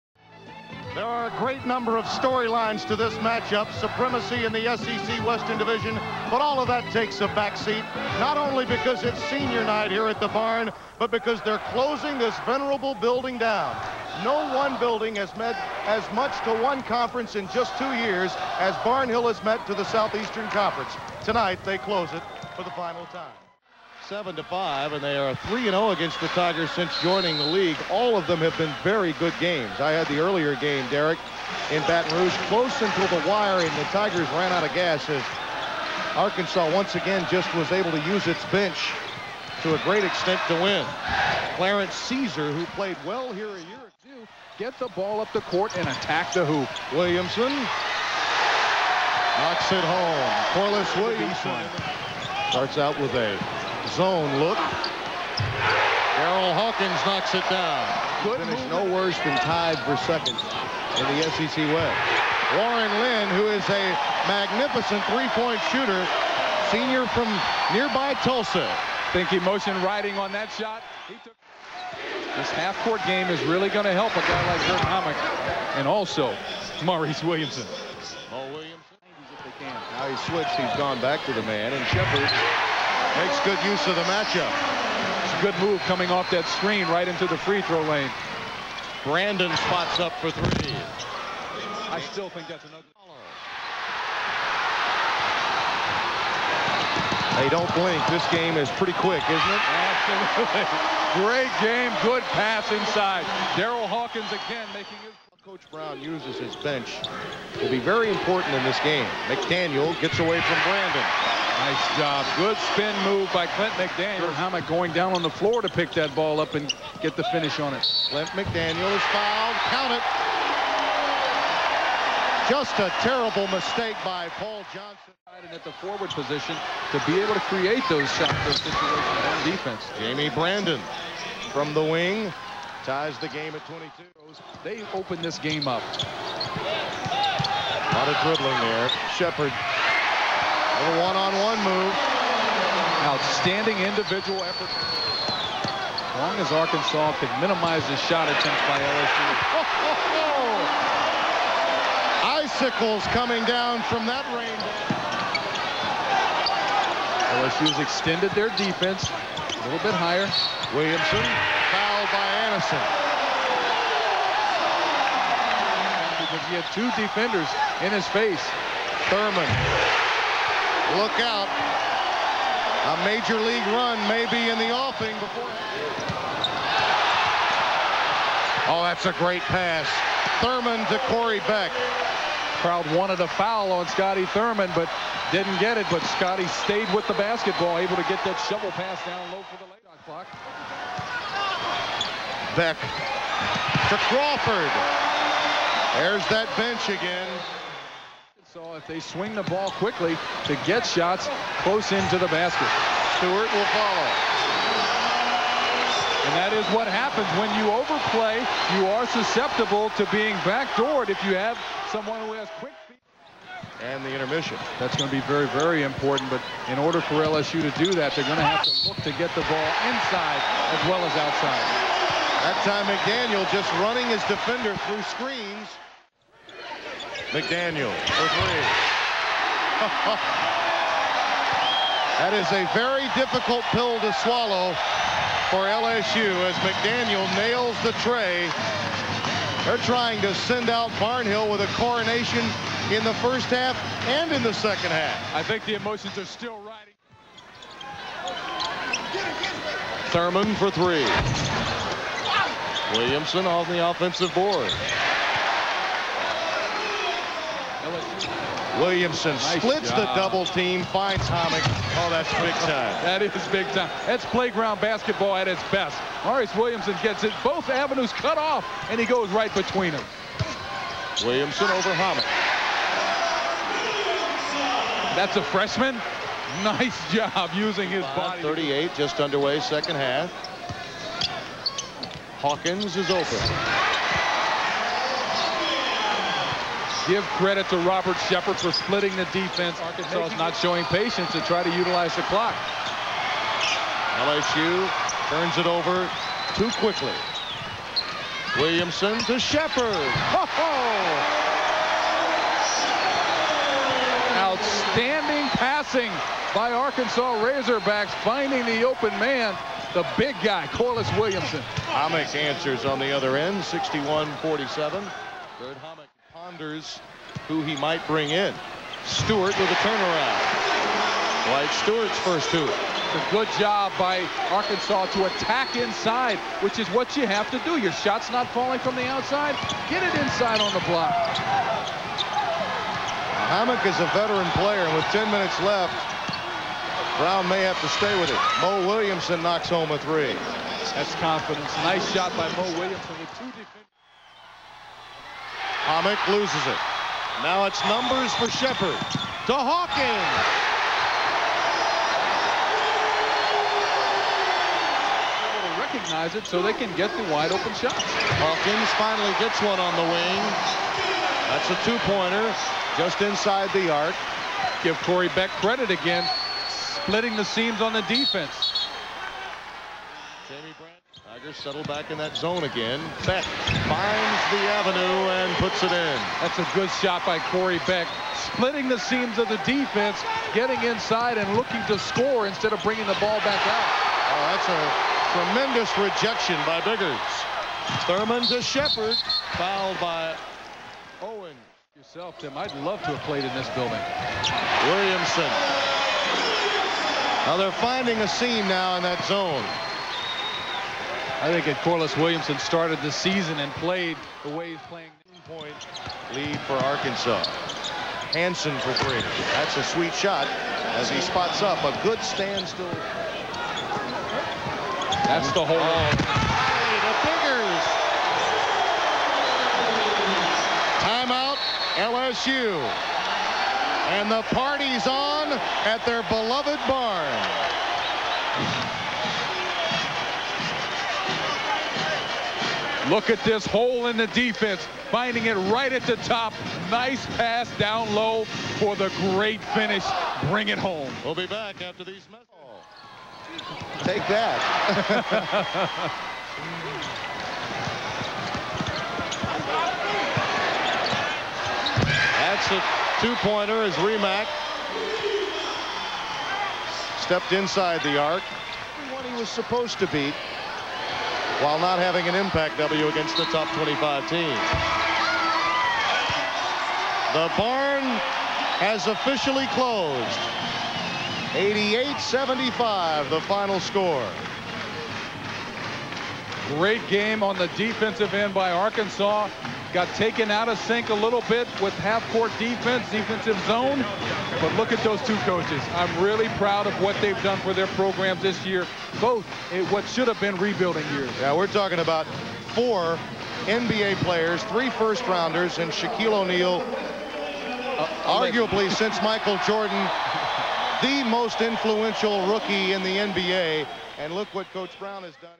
#13 Arkansas vs. LSU 3/3/1993 (Last game at Barnhill Arena)
Highlights of the final game at historic Barnhill Arena.  The #13 Razorbacks would build a commanding 51-34 halftime lead and would cruise to an 88-75 win to clinch their second of four straight SEC West Division Titles.